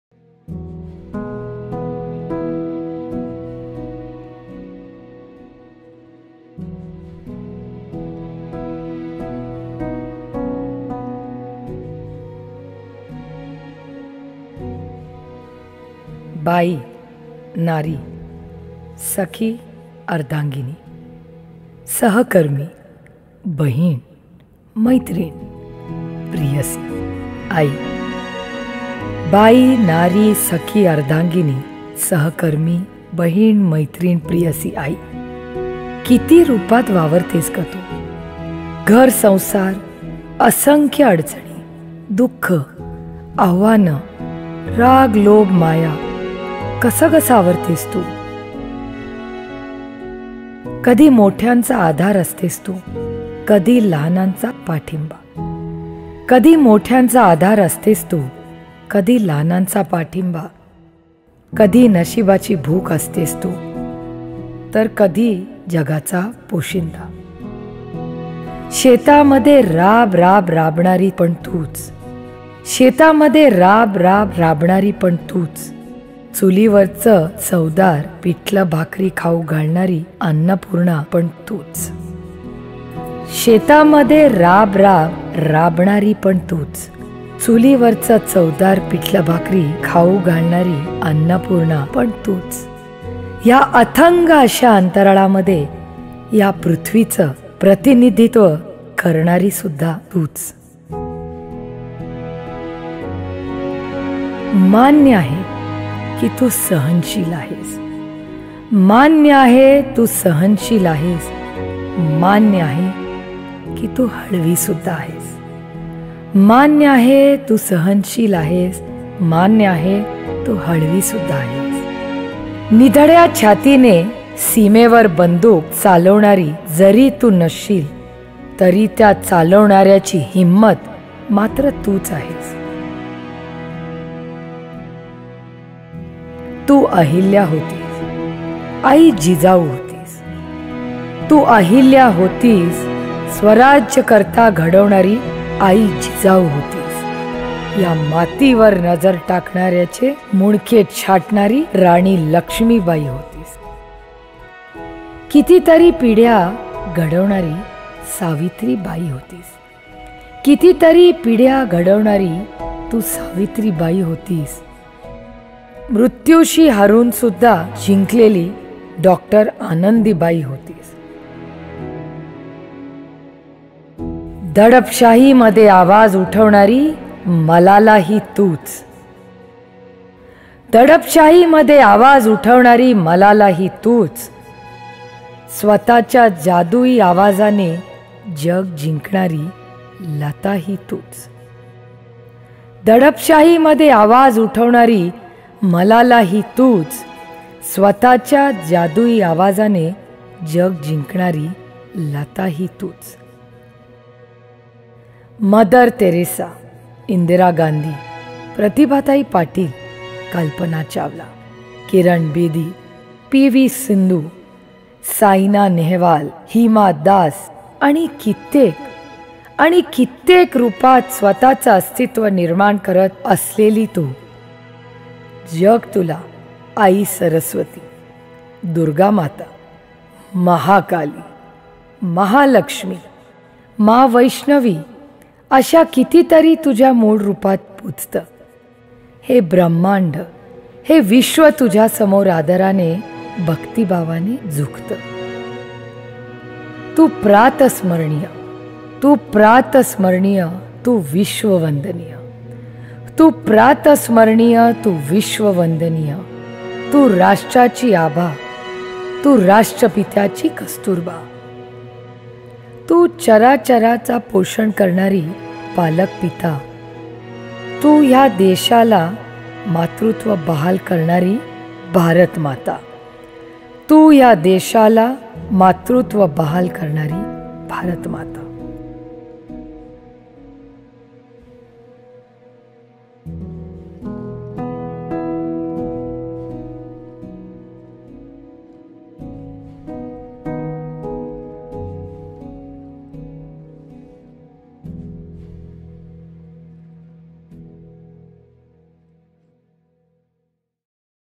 Supriya Sule | नवरात्रोत्सवानिमित्त महिला सक्षमीकरणावर आधारित सुप्रिया सुळेंनी सादर केलेली कविता
Supriya Sule | नवरात्रोत्सवानिमित्त महिला सक्षमीकरणावर आधारित सुप्रिया सुळेंनी सादर केलेली सुंदर कविता